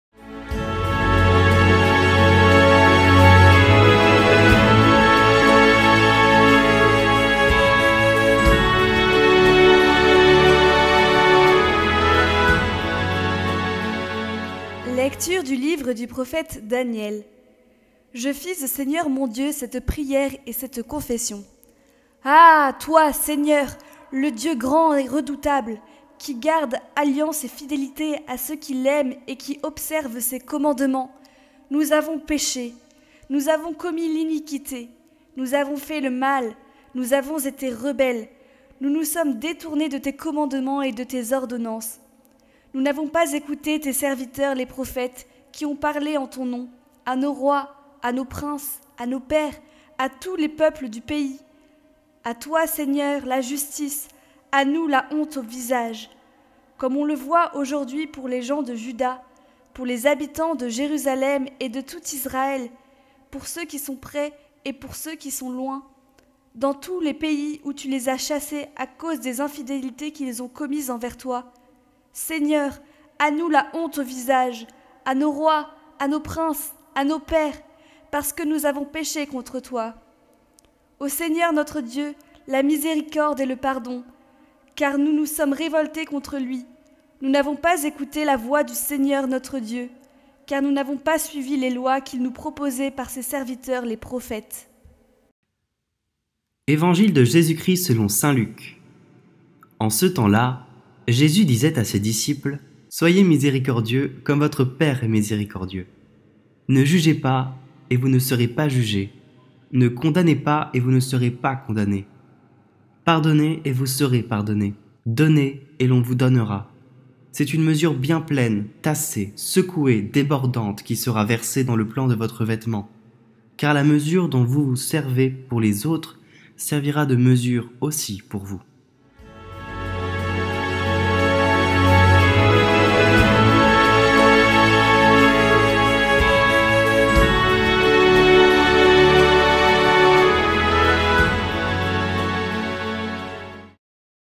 Parole et Evangile chaque Jour | Lundi 18 Mars
PREMIÈRE LECTURE
« Nous avons péché, nous avons commis l’iniquité » (Dn 9, 4-10)
Lecture du livre du prophète Daniel

Je fis au Seigneur mon Dieu cette prière et cette confession :
« Ah ! toi Seigneur, le Dieu grand et redoutable,
qui garde alliance et fidélité
à ceux qui l’aiment et qui observent ses commandements,
nous avons péché,
nous avons commis l’iniquité,
nous avons fait le mal,
nous avons été rebelles,
nous nous sommes détournés
de tes commandements et de tes ordonnances.
Nous n’avons pas écouté tes serviteurs les prophètes,
qui ont parlé en ton nom
à nos rois, à nos princes, à nos pères,
à tout le peuple du pays.
À toi, Seigneur, la justice ;
à nous la honte au visage,
comme on le voit aujourd’hui pour les gens de Juda,
pour les habitants de Jérusalem et de tout Israël,
pour ceux qui sont près et pour ceux qui sont loin,
dans tous les pays où tu les as chassés,
à cause des infidélités qu’ils ont commises envers toi.
Seigneur, à nous la honte au visage,
parce que nous avons péché contre toi.
Au Seigneur notre Dieu, la miséricorde et le pardon,
car nous nous sommes révoltés contre lui,
nous n’avons pas écouté la voix du Seigneur, notre Dieu,
car nous n’avons pas suivi les lois
qu’il nous proposait par ses serviteurs les prophètes. »

PSAUME
(78 (79), 5a.8, 9, 11.13ab)
R/ Seigneur, ne nous traite pas
selon nos péchés. (cf. 102, 10a)

Combien de temps, Seigneur, durera ta colère ?
Ne retiens pas contre nous les péchés de nos ancêtres :
que nous vienne bientôt ta tendresse,
car nous sommes à bout de force !

Aide-nous, Dieu notre Sauveur,
pour la gloire de ton nom !
Délivre-nous, efface nos fautes,
pour la cause de ton nom !

Que monte en ta présence la plainte du captif !
Ton bras est fort : épargne ceux qui doivent mourir.
Et nous, ton peuple, le troupeau que tu conduis,
sans fin nous pourrons te rendre grâce.

ÉVANGILE
« Pardonnez, et vous serez pardonnés » (Lc 6, 36-38)
Gloire à toi, Seigneur,
Fils du Dieu vivant !
Tes paroles, Seigneur,
sont esprit et elles sont vie.
Tu as les paroles de la vie éternelle.
Fils du Dieu vivant ! (cf. Jn 6, 63c.68c)

Jésus disait à ses disciples :
« Soyez miséricordieux comme votre Père est miséricordieux.
Ne jugez pas, et vous ne serez pas jugés ;
ne condamnez pas, et vous ne serez pas condamnés.
Pardonnez, et vous serez pardonnés.
Donnez, et l’on vous donnera :
c’est une mesure bien pleine, tassée, secouée, débordante,
qui sera versée dans le pan de votre vêtement ;
car la mesure dont vous vous servez pour les autres
servira de mesure aussi pour vous. »